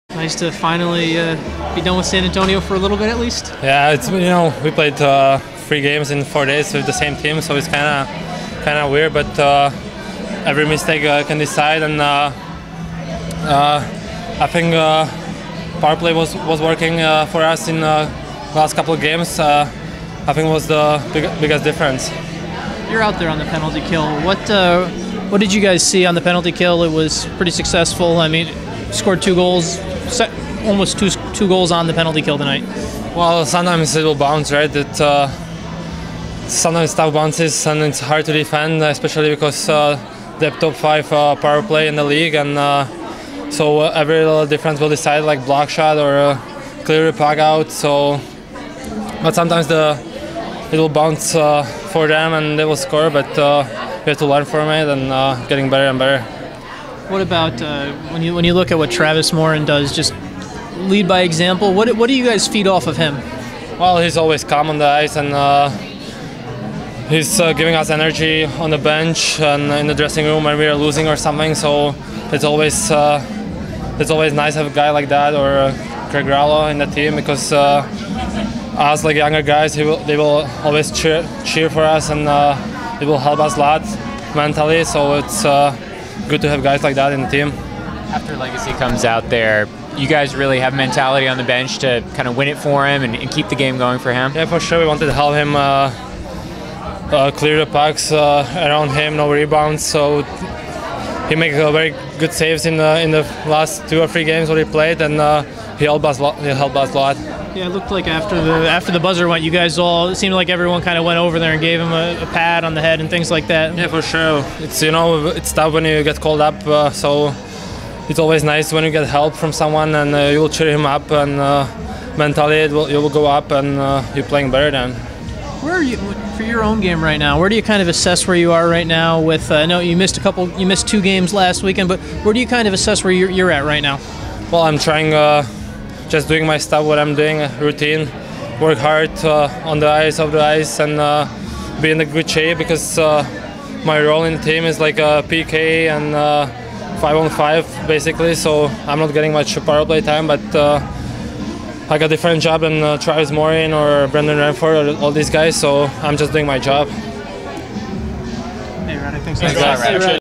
Postgame Interview: Radek Faksa, 12/29/2015
Radek Faksa talks with media about the team's performance in a txstars 5-4 OT win against the San Antonio Rampage.